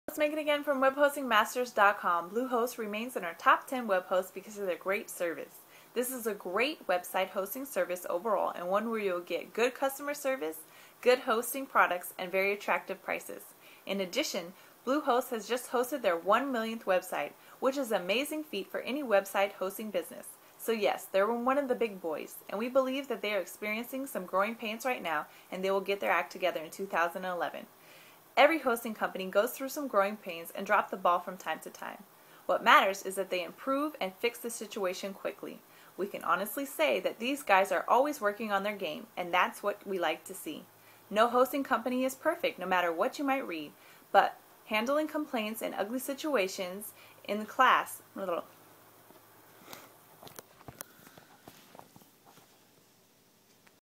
Bloggers Love BlueHost - Find Out Why with our BlueHost Review
The WebHostingMasters BlueHost review from Megan.  Sexy, smart, and a hosting guru, she provides you with a quick review of this top company.  Here is our full BlueHost Review: